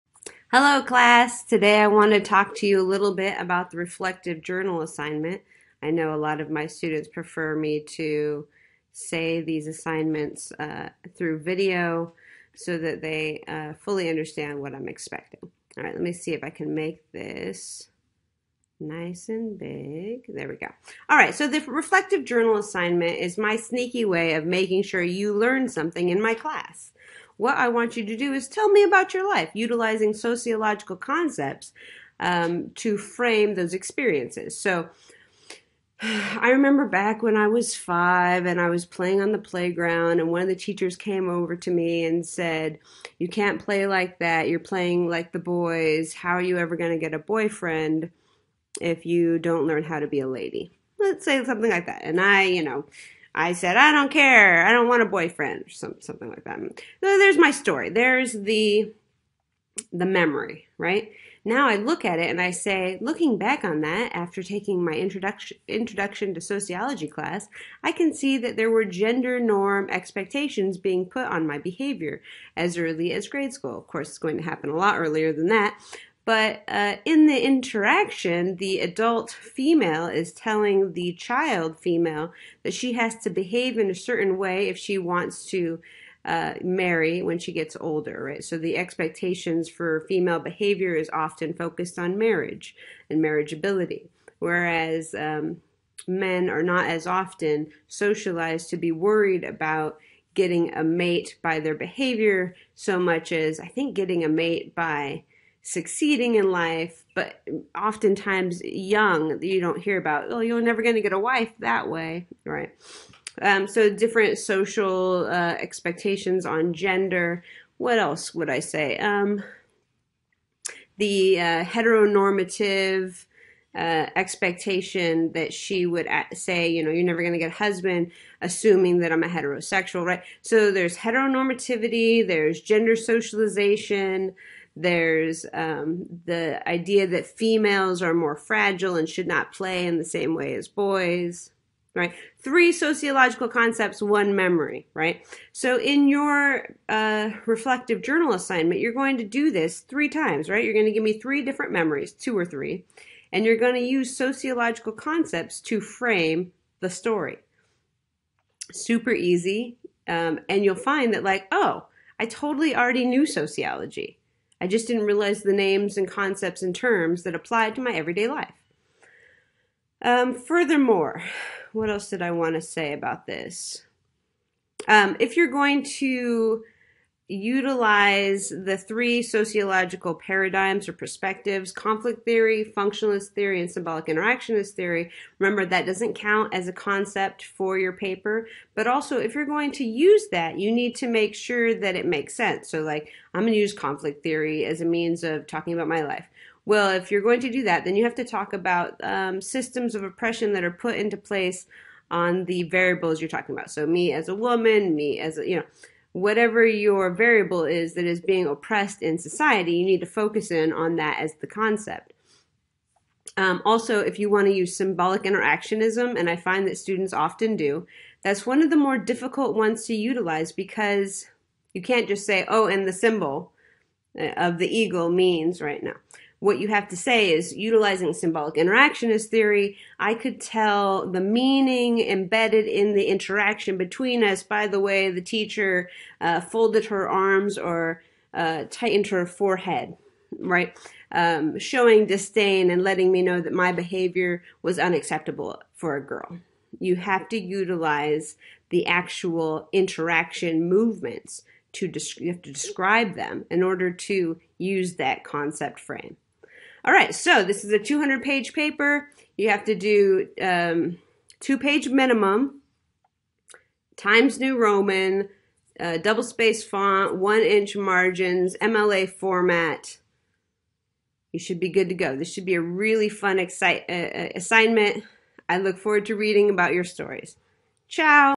Reflective Journal Assignment
A video on the Reflective Journal Assignment..
please ignore the 200 page paper mishap! I meant 200 point paper, 2 page minimum